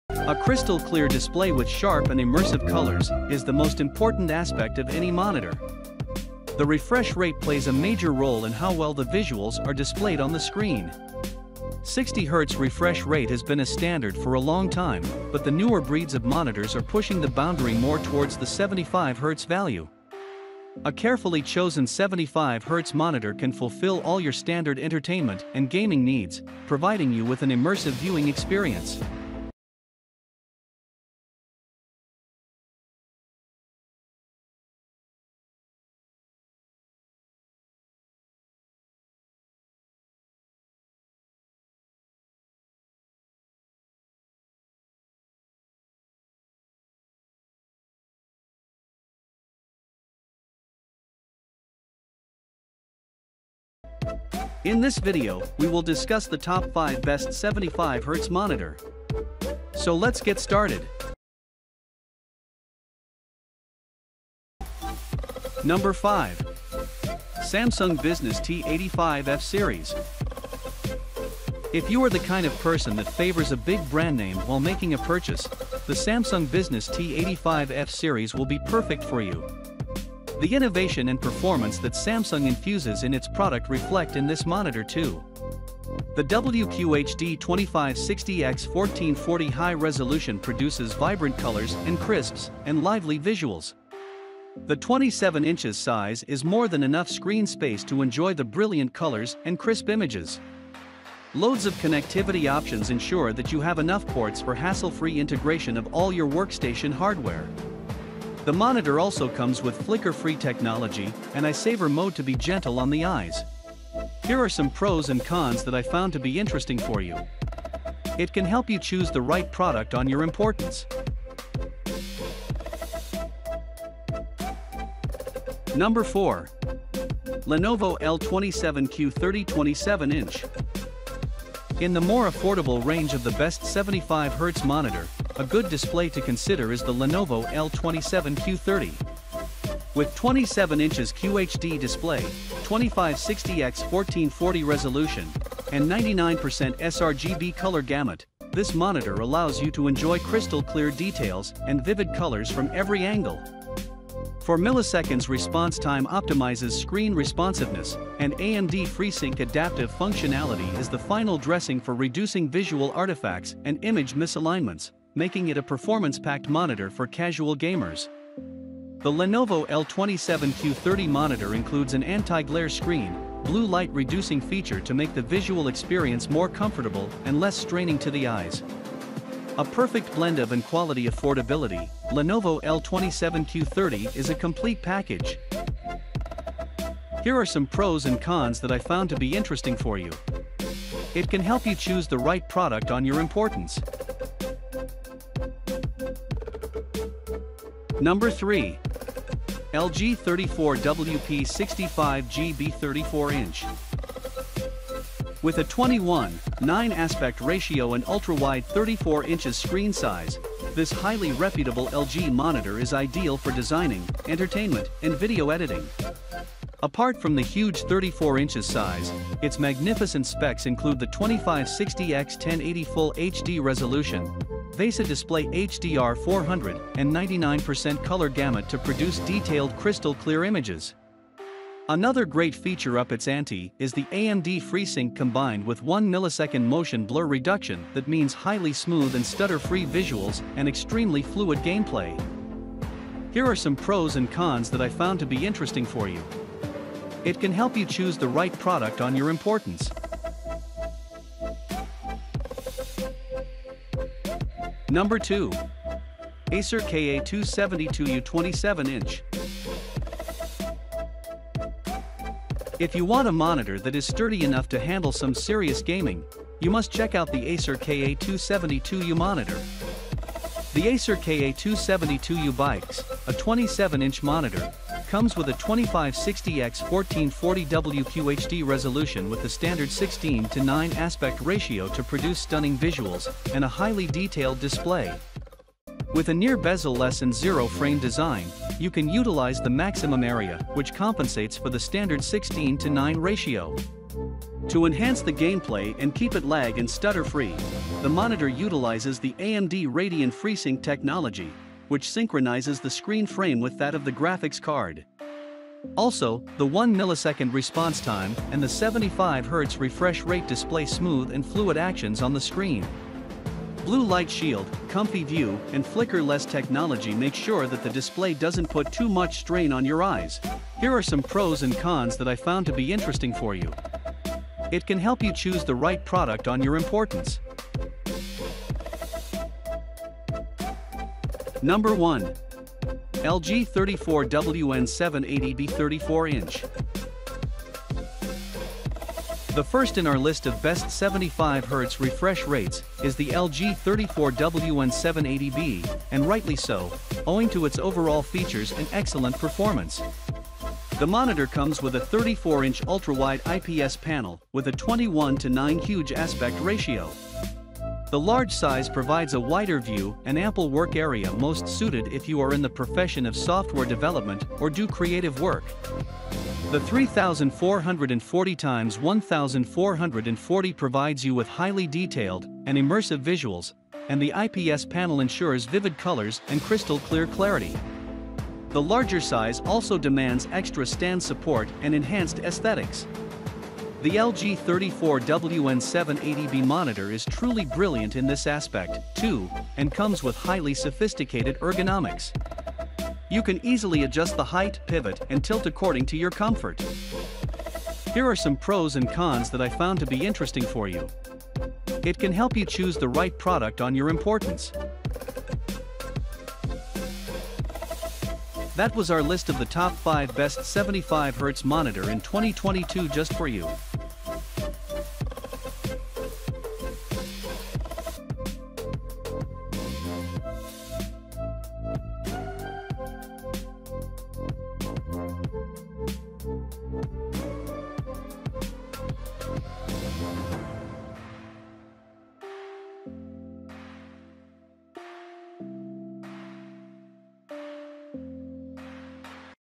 Top 5 Best 75 Hz Monitor
Here we are talking about top 5 best 75 hz monitor in 2024. If you have been looking for the best monitor with high refresh rate of 75 Hz, then you don’t need to worry anymore because here we have brought you some electronic gadgets which can help you increase your visual performance. As per today’s technological trends, people are using the 75 Hz monitors for their daily use. These 75 Hz monitors are the best option for gamers and professionals as well. Do keep reading till the end and find out more interesting things about them.

Best  75 Hz Monitor In 2022:

00:00 - Intro

1:07 - 5. Samsung Business T85F Series

2:08 - 4. Lenovo L27q-30

3:20 - 3. LG 34WP65G-B

4:16 - 2. Acer KA272U biipx

5:33 - 1. LG 34WN780-B